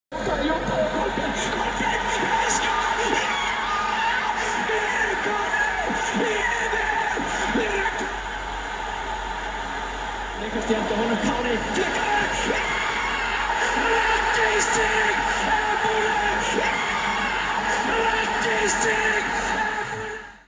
ISLAND TV-REPORTER 2:1 England GUDMUNDUR BENEDIKTSSON
Er ist DER TV-Reporter der Euro 2016 und freut sich über das 2:1 gegen England.